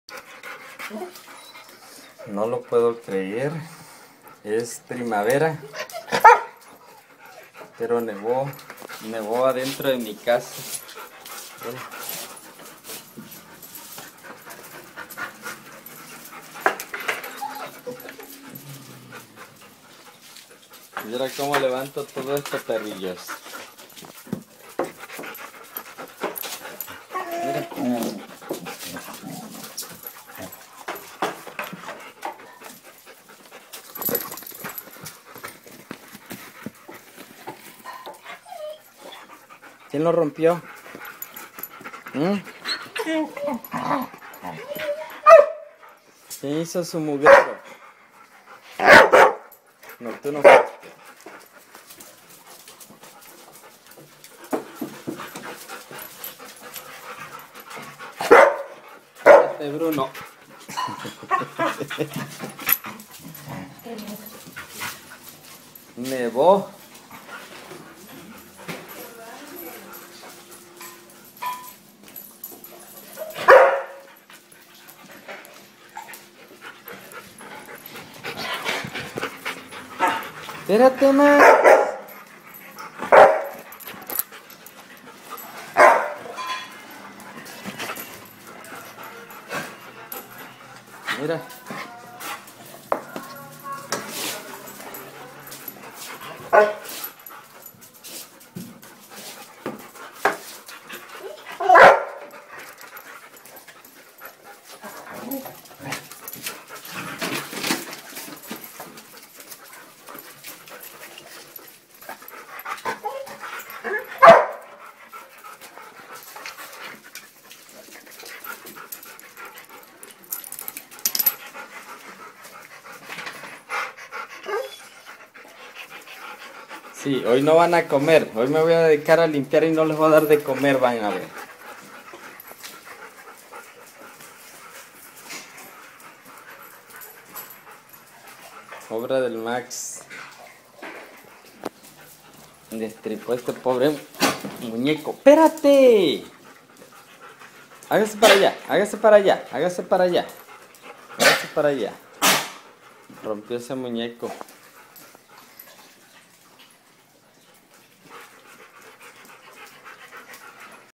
Neva en Primavera.
Una sorpresa al abrir la puerta del cuarto, había nieve por todo el resto de la casa, obra del Max.